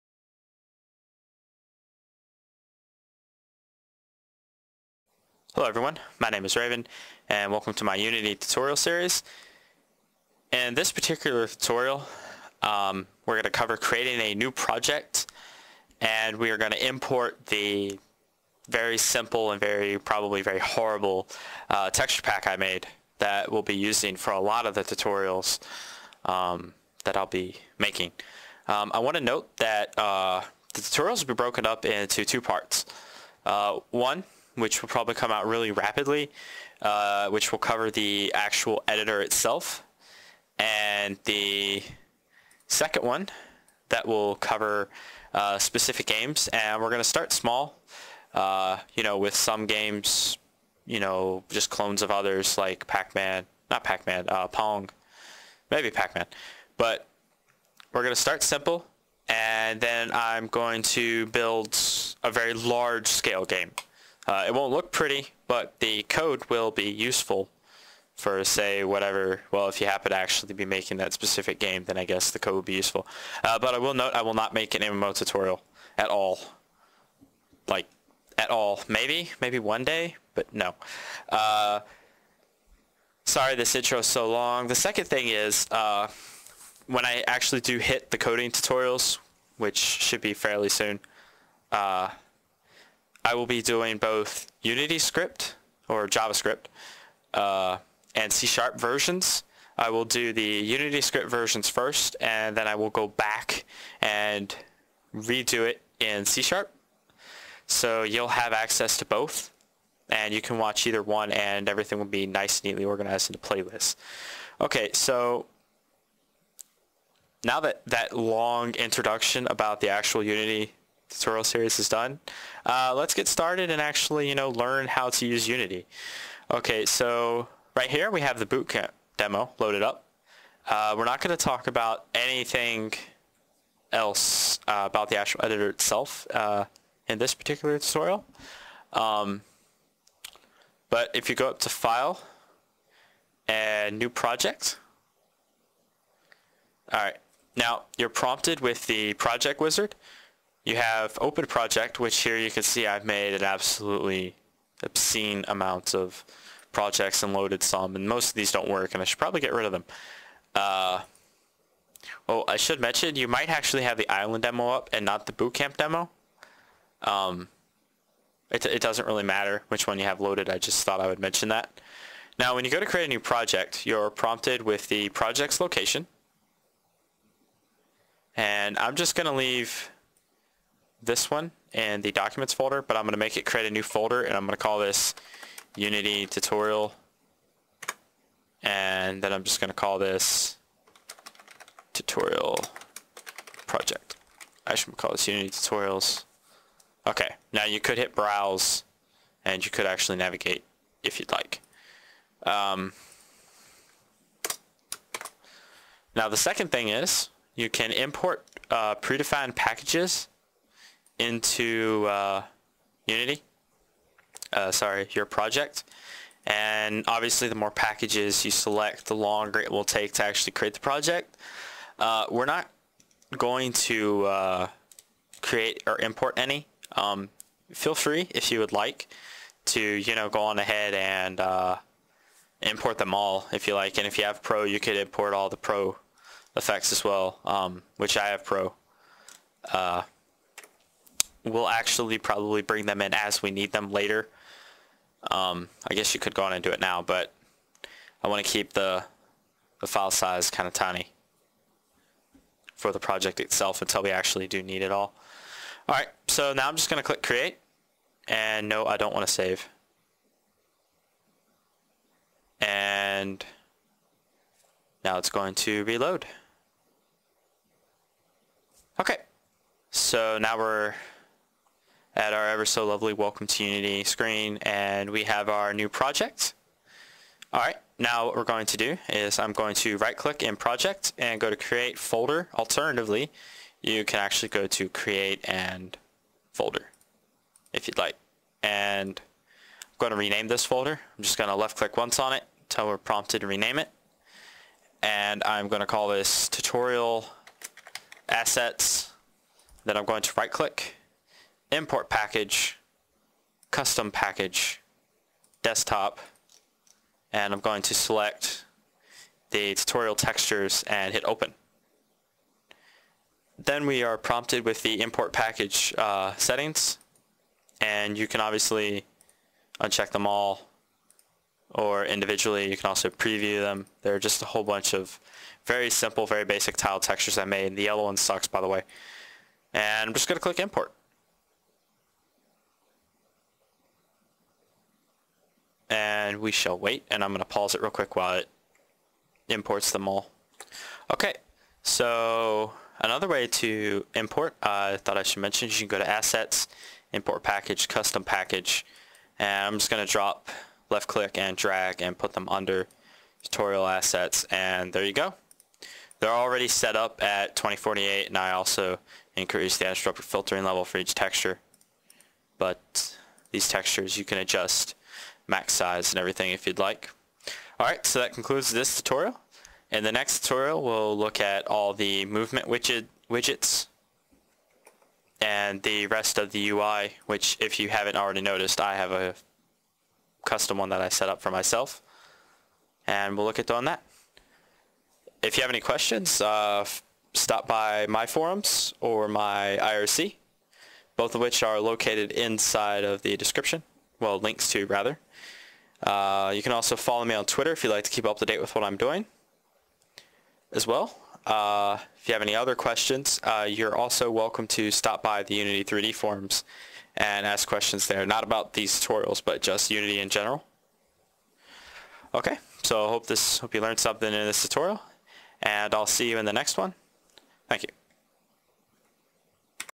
Unity3D Editor Tutorial Series: - Creating Our First Project
Unity3D tutorial showing users how to create there first project.